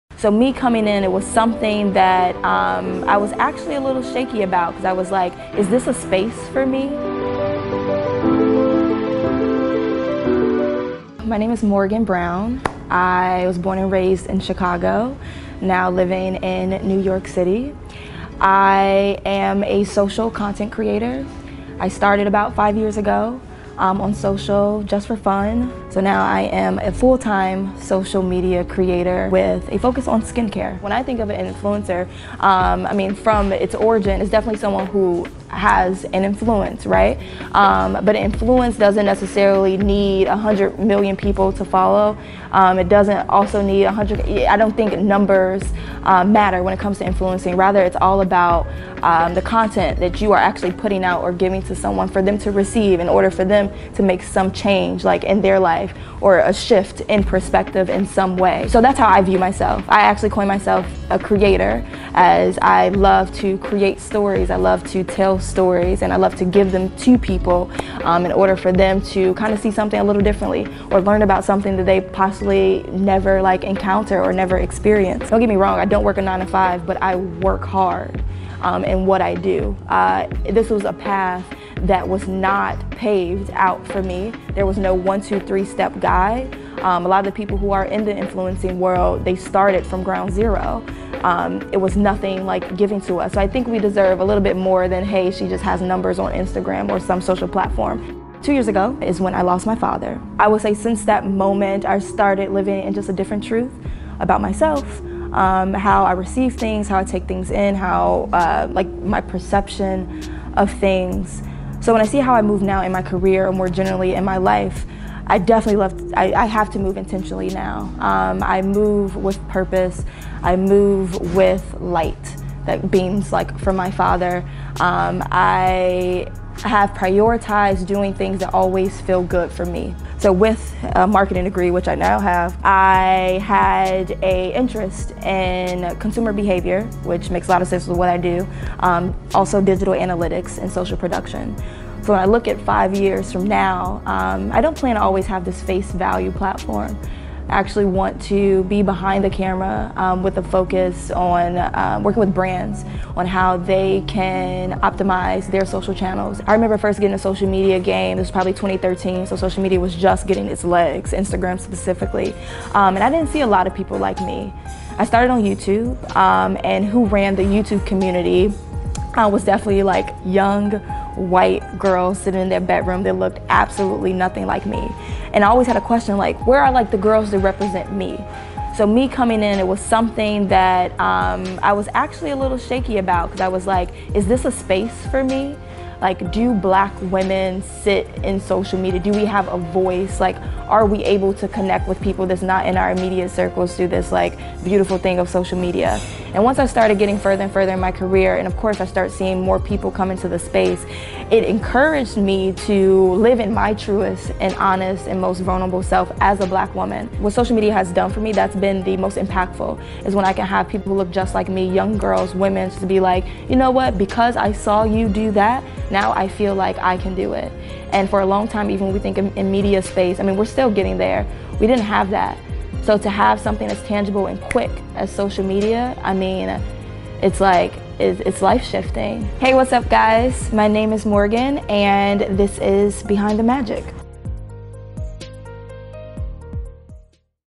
Behind The Magic: Morgan Brown
GU gets behind the scenes of NY based Content Creator, Morgan Brown.